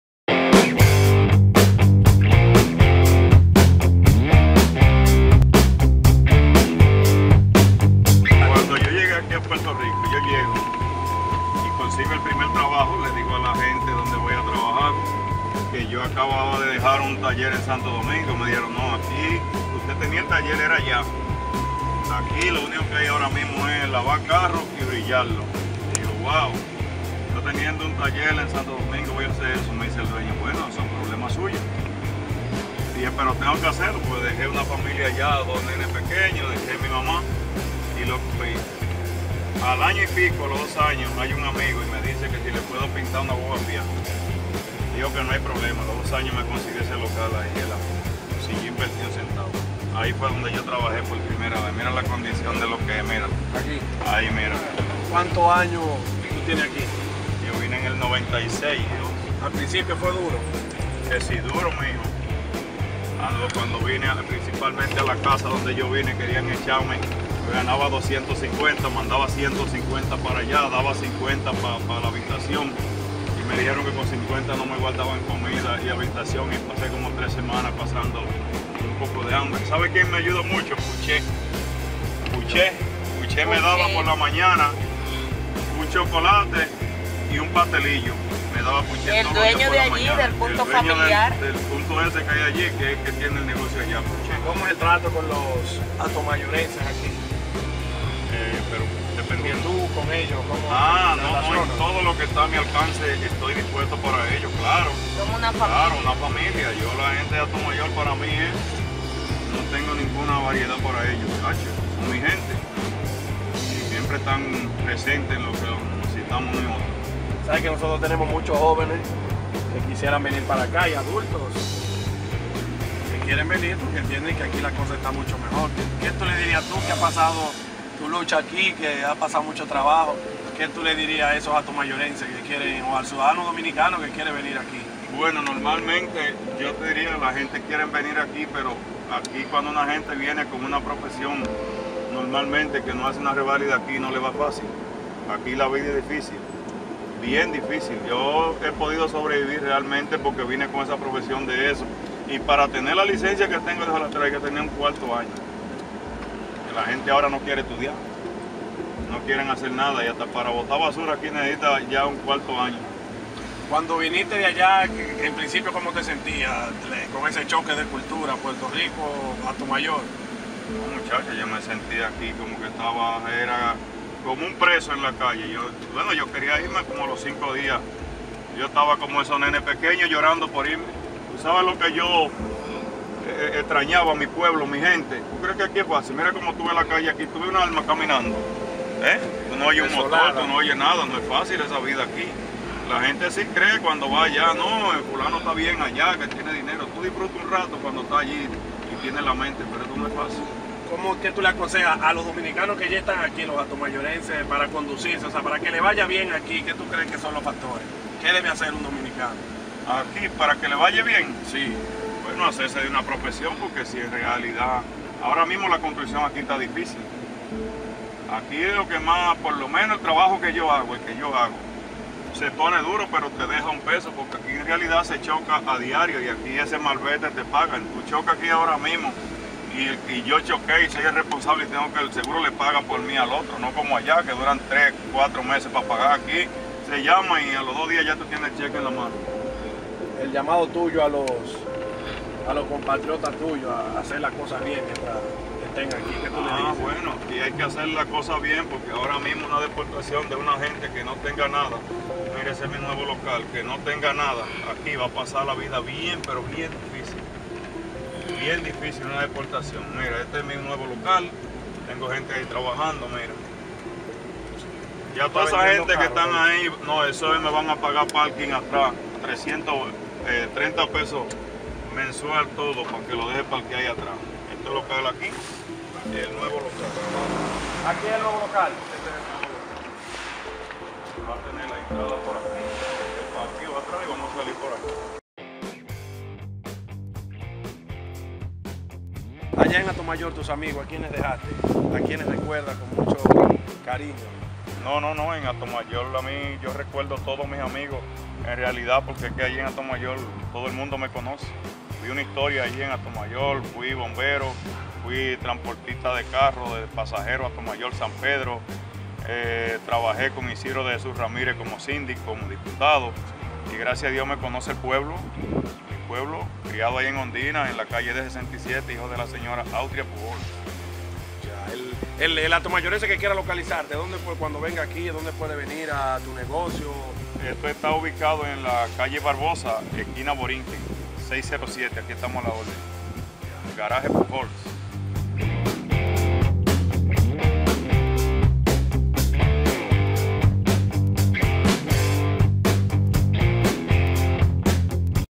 Manuel Candelario 'Tachi', hatomayorense residente en Puerto Rico
Conozca la historia de Manuel Candelario 'Tachi', un hatomayorense residente en Puerto Rico, que representa la superación y el trabajo dedicado.